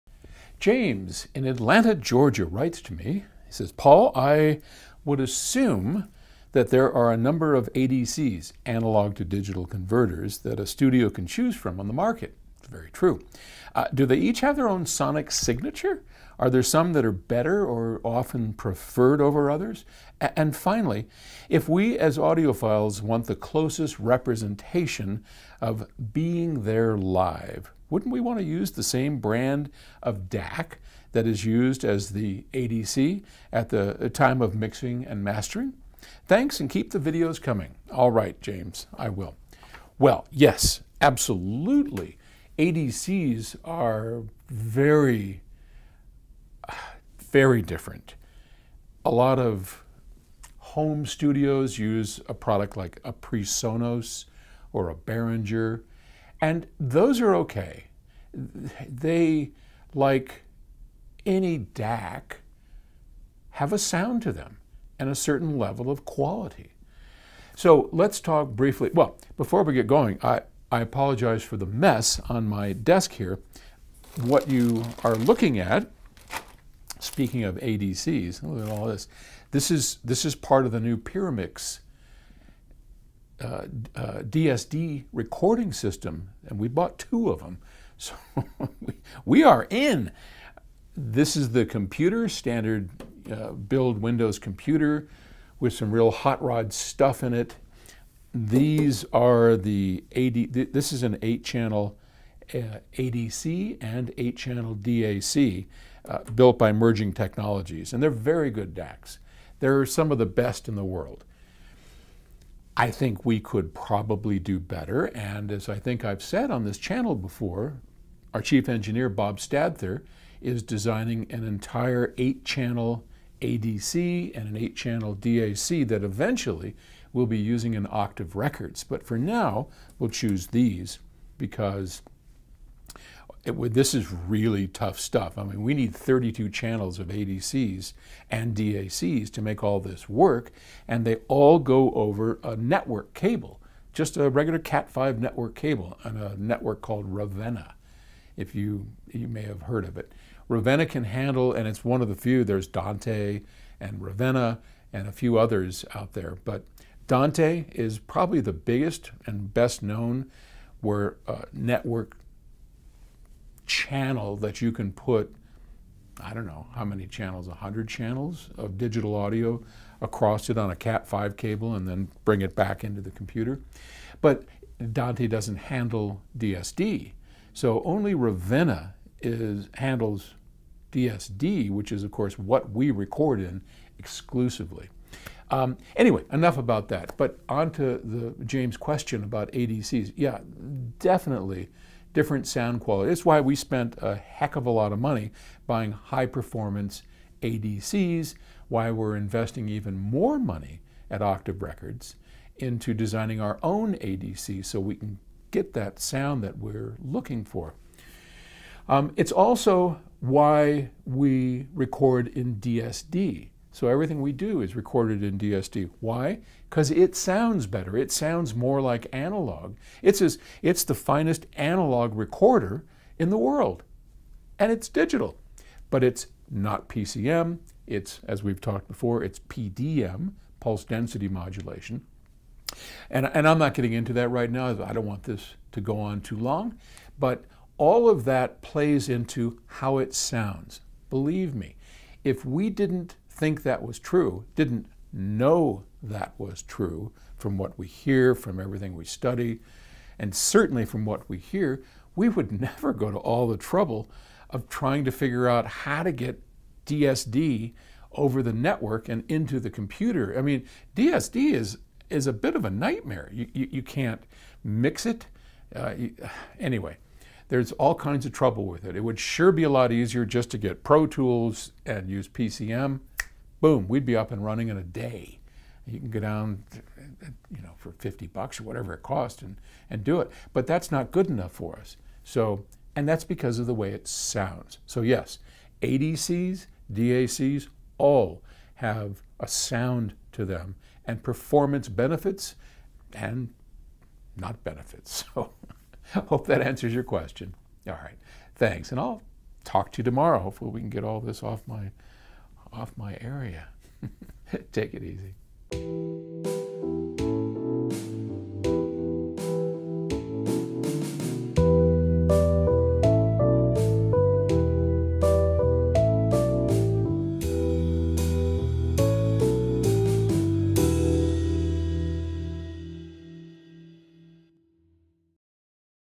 Do ADCs have a sound
Analog to digital converters are the heart of a modern recording studio. How important are they in the recording chain?